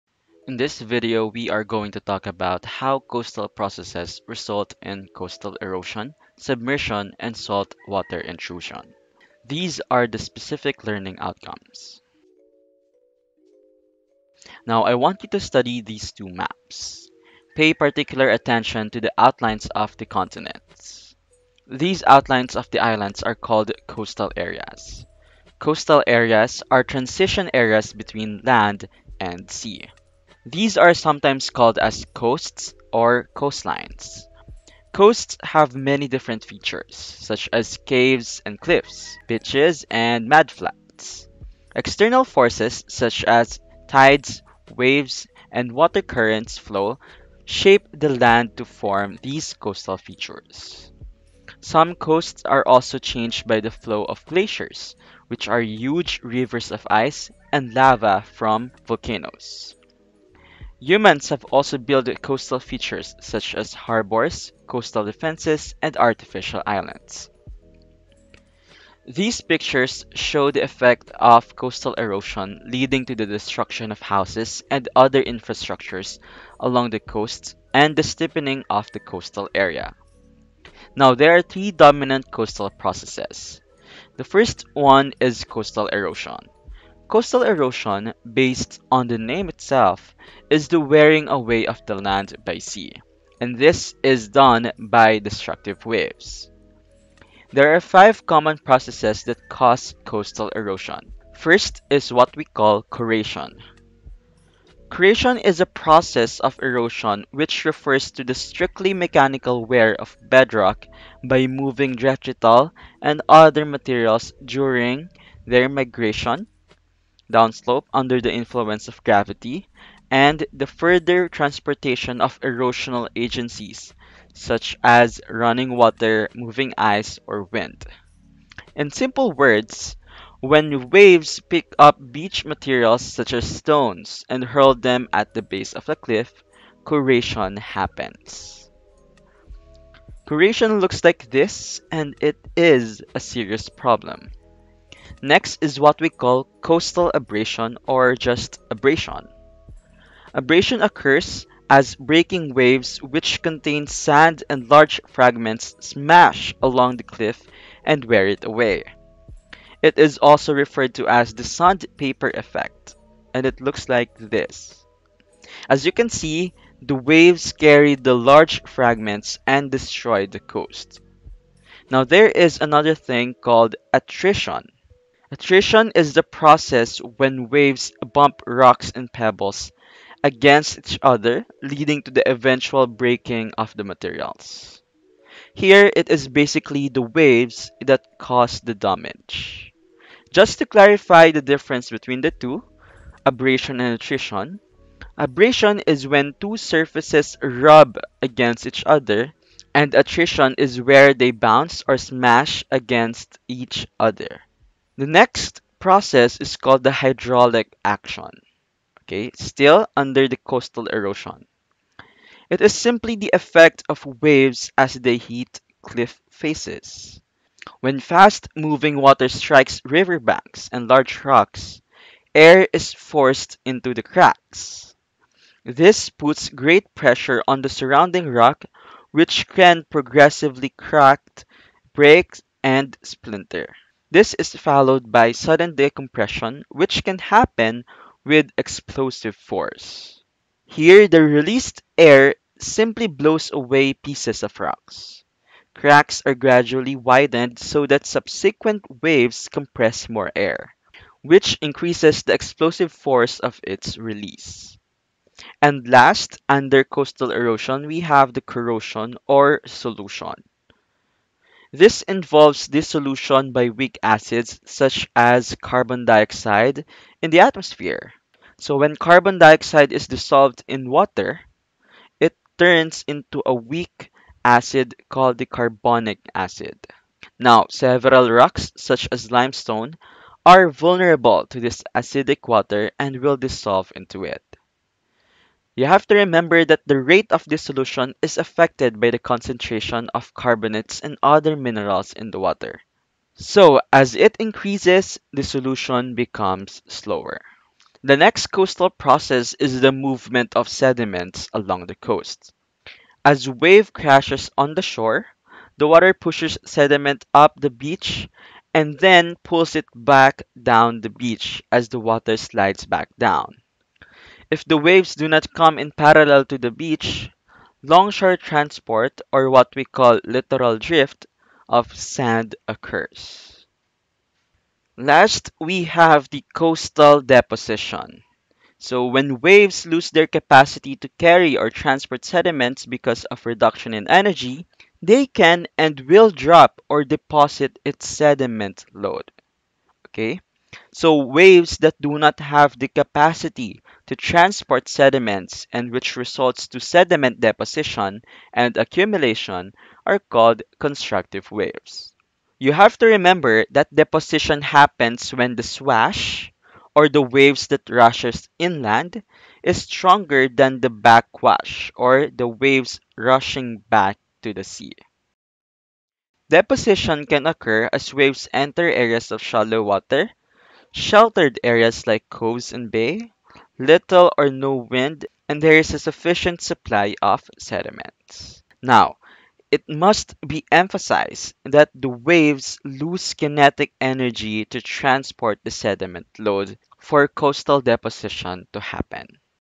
How Coastal Processes Result in Coastal Erosion, Submersion, and Saltwater Intrusion
Natural Hazards, Mitigation, and Adaptation: Marine and Coastal Processes and Their Effects

Learning Competency
The learners will be able to describe how coastal processes result in coastal erosion, submersion, and saltwater intrusion

Specific Learning Outcomes
At the end of the lesson, the learners will be able to:
1. Recognize the coastal processes that influence the coastal landforms and associated hazards
2. Illustrate and describe how the coastal processes determine the present coastal hazards whether coastal erosion, submersion, or saltwater intrusion

Notes:
- Coastal Areas are transition areas between land and sea
- Coasts have many different features such as caves and cliffs, beaches, and mudflats.
- External forces such as tides, waves, and water currents (flow) shape the land to form these coastal features.
- Coastal Erosion is the wearing away of the land by the sea.

Five Common Processes that Cause Coastal Erosion
1. Corrasion - Corrasion is a process of erosion which refers to the strictly mechanical wear of bedrock by moving detrital and other materials during (a) their migration downslope under the influence of gravity, and (b) their further transportation by erosional agencies such as running water, moving ice, or wind.
2. Abrasion - Abrasion occurs as breaking waves which contain sand and large fragments smash along the cliff and wear it away
3. Attrition - Attrition is the process when waves bump rocks and pebbles against each other leading to the eventual breaking of the materials.
4. Hydraulic Action - It is simply the effect of waves as they hit cliff faces.  When fast-moving water strikes riverbanks and large rocks, air is forced into cracks.  This puts great pressure on the surrounding rock, which can progressively crack, break, and splinter. This is followed by sudden decompression, which can happen with explosive force.
5. Corrosion / Solution - This involves dissolution by weak acids such as when the carbon dioxide in the atmosphere is dissolved into water turning it into a weak carbonic acid.